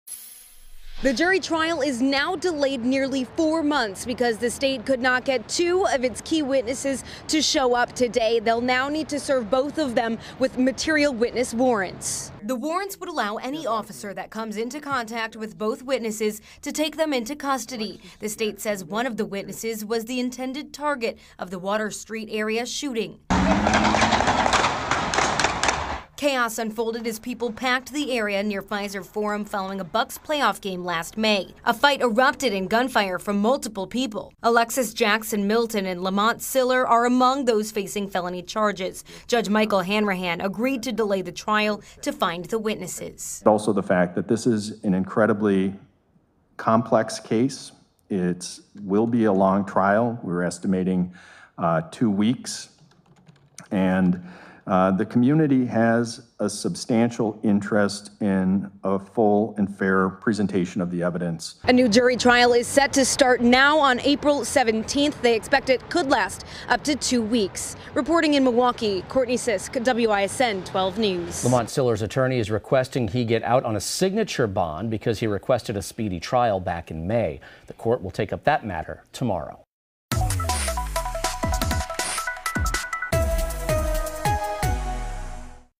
Trial delayed for two charged in Bucks playoff shooting
Alexus Jackson Milton and Lemont Siller both face 16 counts of first-degree reckless injury for their alleged involvement in a shooting that left 17 people shot near Water Street and Juneau Avenue.